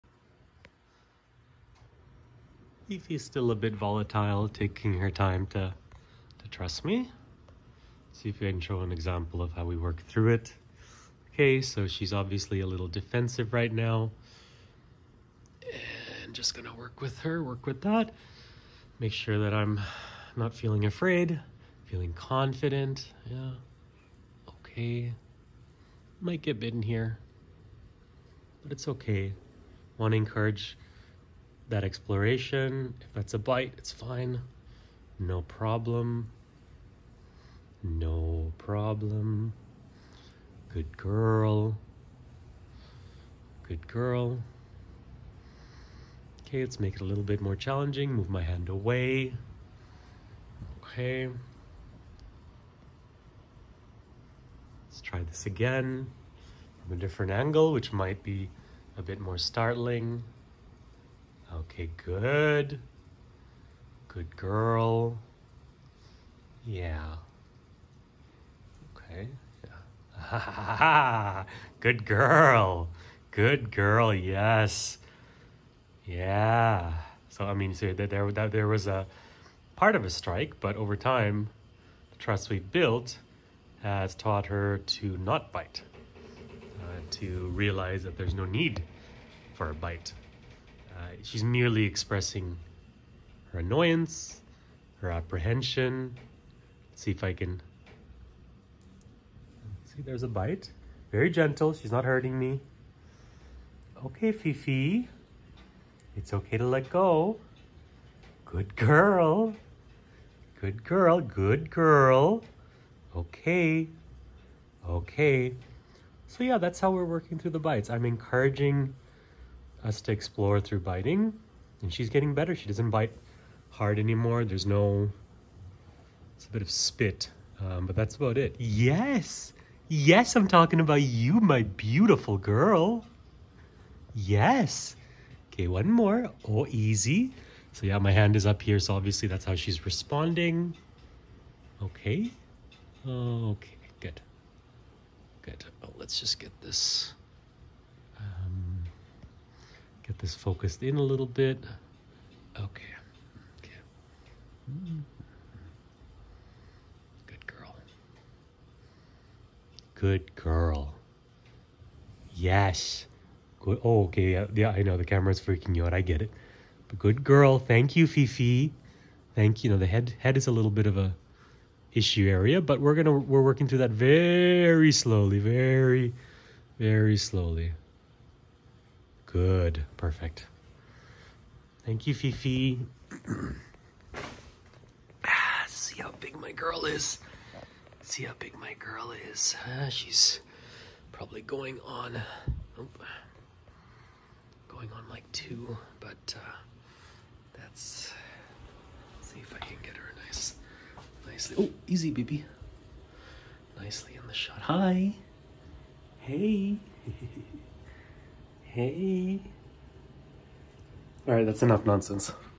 Working through biting with Phoenix, my Asian King Rat Snake.
Fifi can be really bitey, especially when she feels threatened or overwhelmed, and that can happen very easily. I work with her regularly to build trust and socialize her. I encourage biting, of sorts, and do my best to work through it with her. Can’t wait for her to be a big wonderful monster!